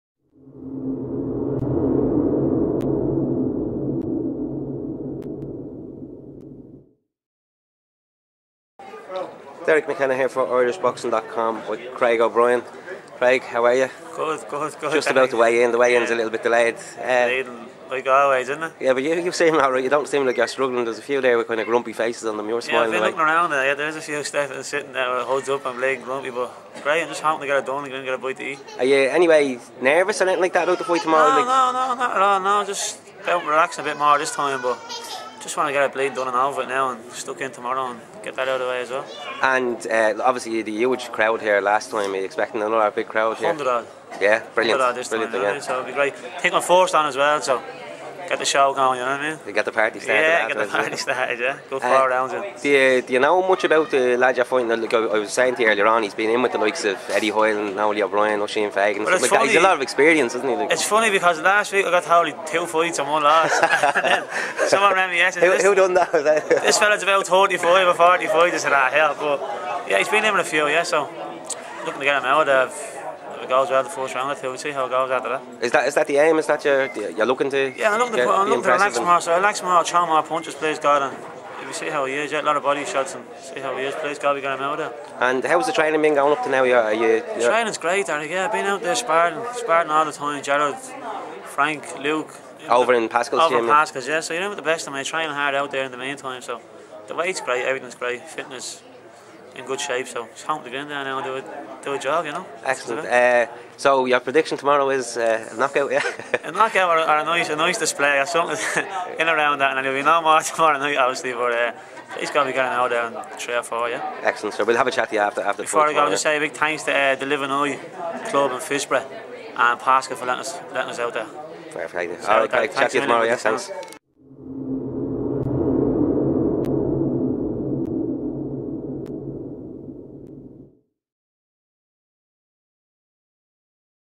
Craig O'Brien: I'll be looking to get him out in Rd 1 or 2
Craig O'Brien talks to Irish-boxing.com's Derek McKenna ahead of his fight with Asen Vasilev